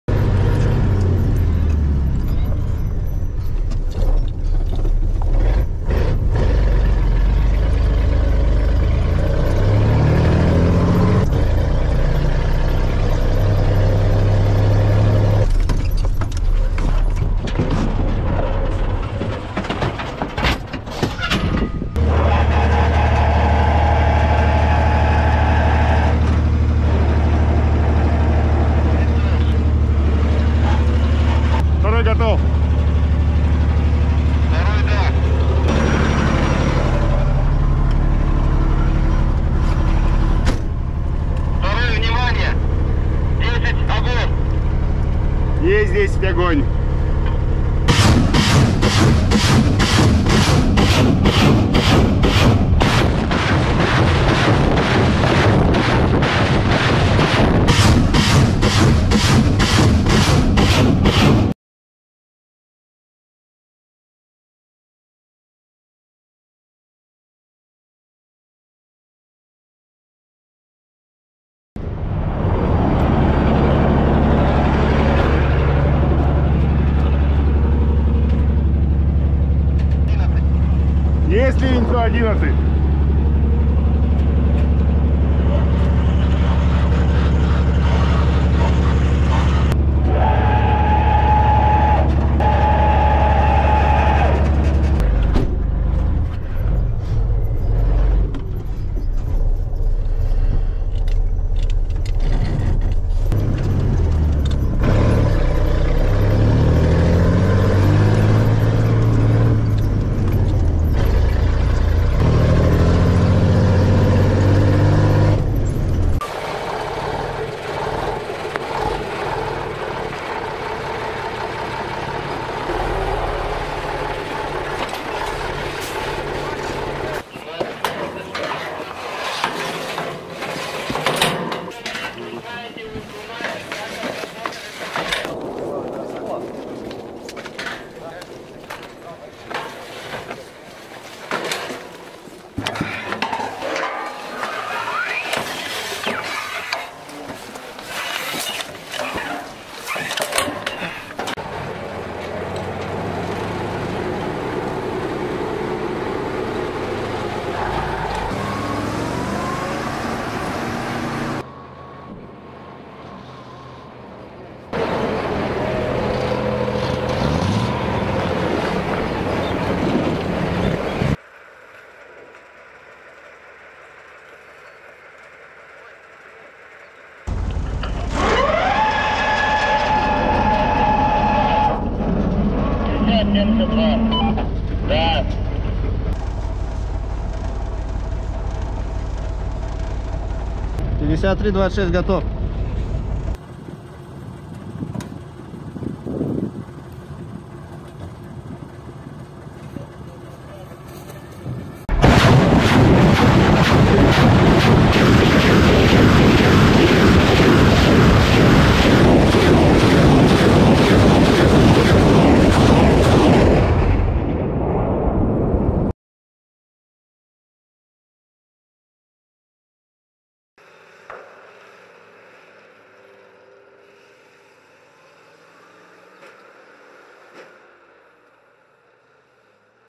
Multiple rocket launchers «Grad» daily destroys equipment and manpower
Multiple rocket launchers «Grad» daily destroys equipment and manpower of the Armed Forces of Ukraine